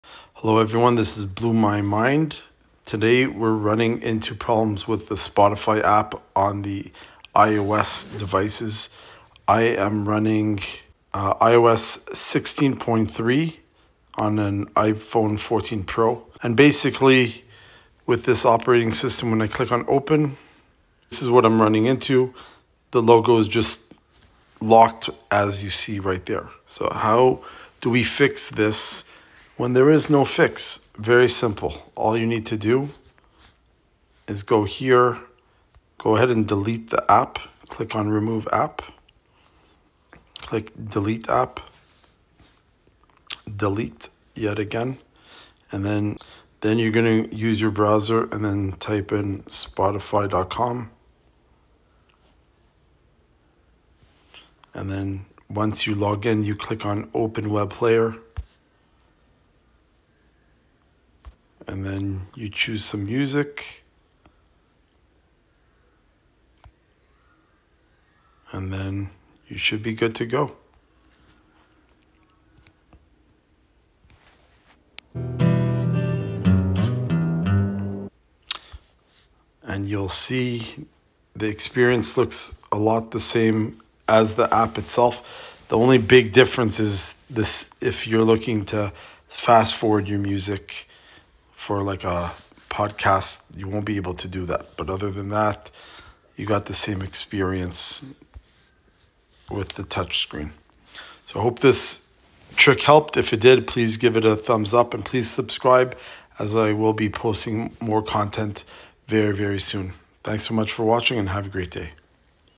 How to fix Spotify bug on Apple IOS 16
If your Spotify isn’t loading on your iOS 16 device, then check out this simple instruction to get your music back in your phone!